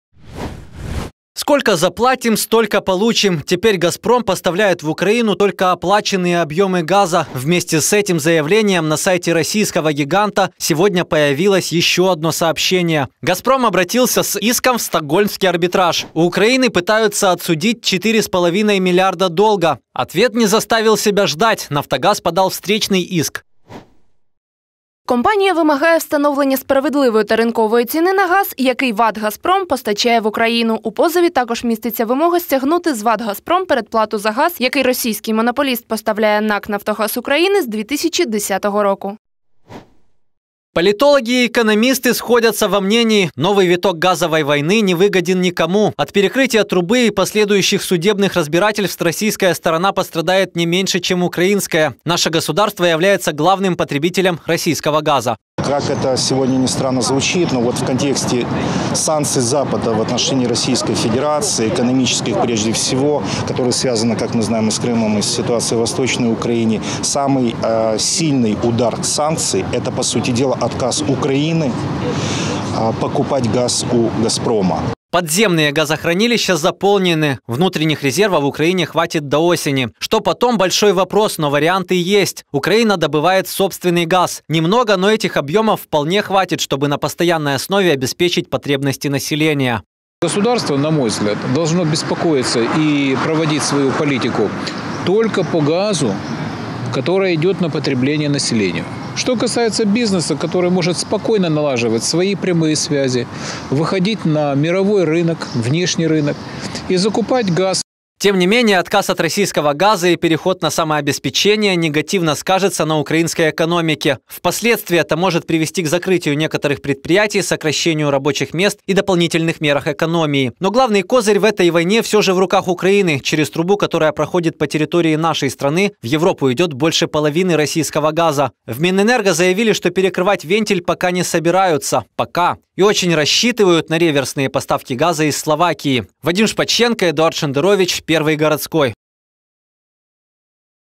ГАЗОВАЯ ВОЙНА
Труба перекрыта! Россия полностью прекращает поставки газ в Украину. Об этом сообщил министр энергетики Юрий Продан. С сегодняшнего дня Газпром перевел Нафтогаз на предоплату. То есть, теперь Украина будет получать столько газа, сколько сможет оплатить и лишь после того, как деньги поступят на счета российского монополиста. О последствиях нового витка газового конфликта, сюжете Вадима Шпаченко.